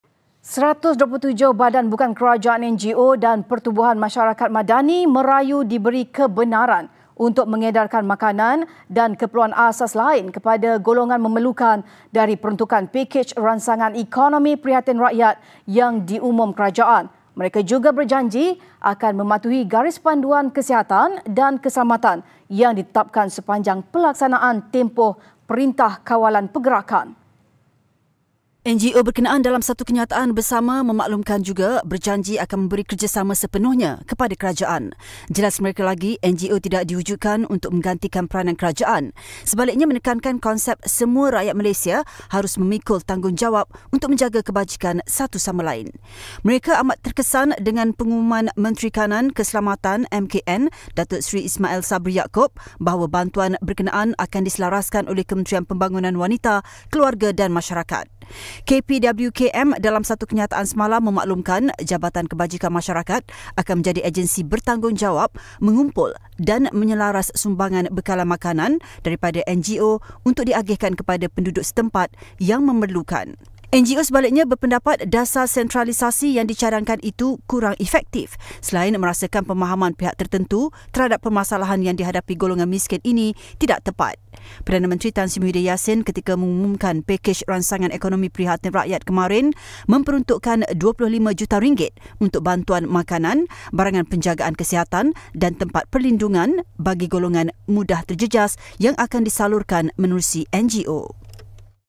PAKEJ PRIHATIN | NGO Rayu Dibenar Edar Bantuan
127 Badan Bukan Kerajaan (NGO) dan Pertubuhan Masyarakat Madani merayu diberi kebenaran untuk mengedarkan makanan dam keperluan asas lain kepada golongan memerlukan dari peruntukan Pakej Rangsangan Ekonomi Prihatin Rakyat yang diumum kerajaan.

Mereka juga berjanji akan mematuhi garis panduan kesihatan dan keselamatan yang ditetapkan sepanjang pelaksanaan tempoh Perintah Kawalan Pergerakan.